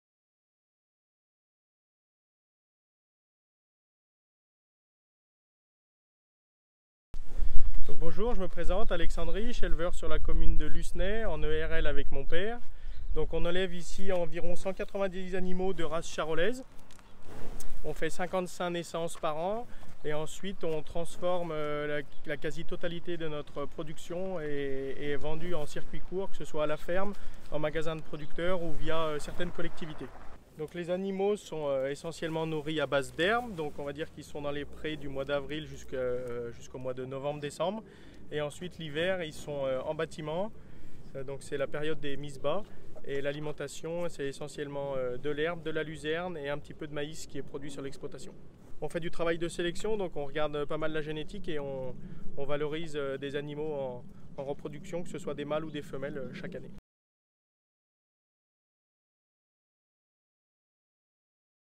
Talents du Rhône 2023 : Canton de Anse
Zoom sur la production de vaches charolaises d'Alexandre Riche, à Lucenay, dans le canton de Anse, talent du Rhône 2023.